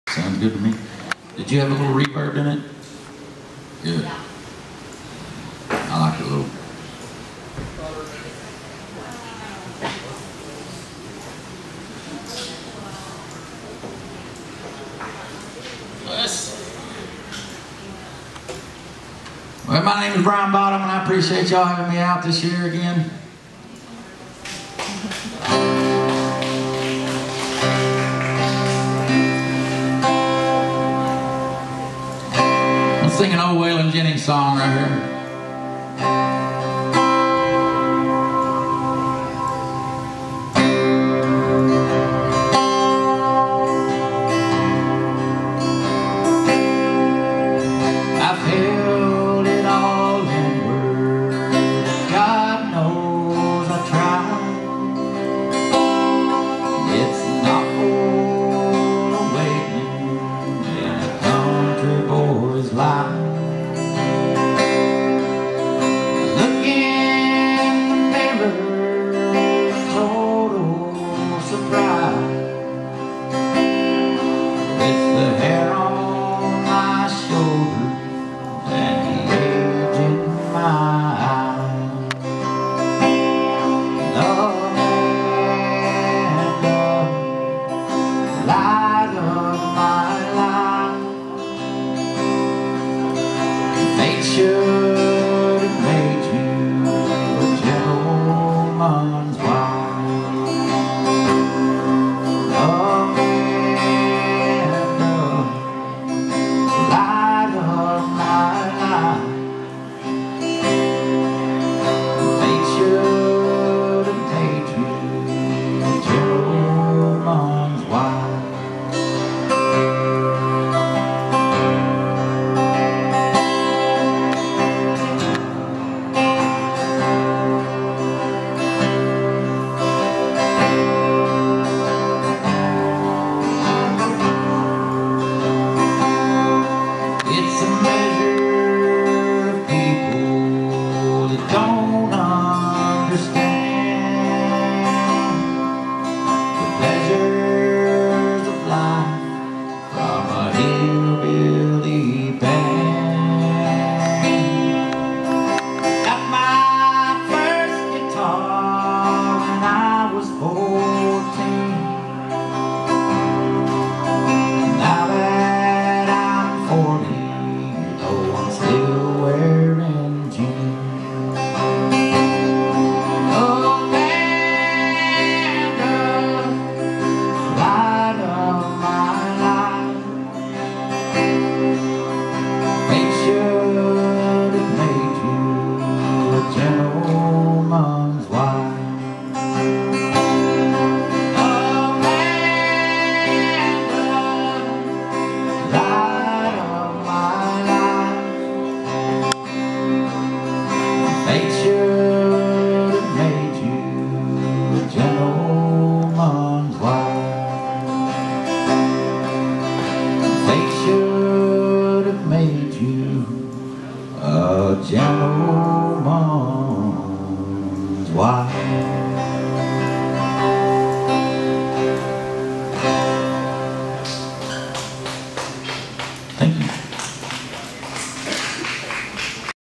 The Native Pride Riders Biketober Jam, Brian Bottom
Brian Bottom playing at The Native Pride Riders Biketober Jam.
Brian Kicked off the Saturday music, for the Biketober Jam event.